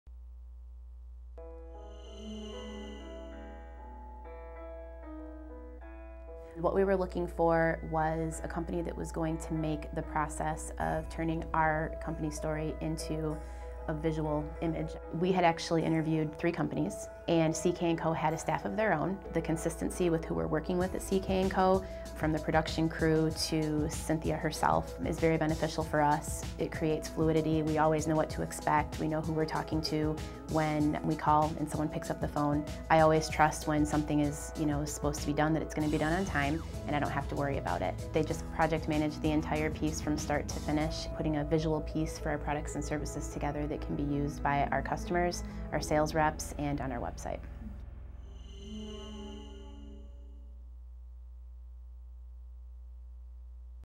US Signal Customer Testimonials
Amanda Regnerus explains what it is like to work with Cynthia Kay and Company.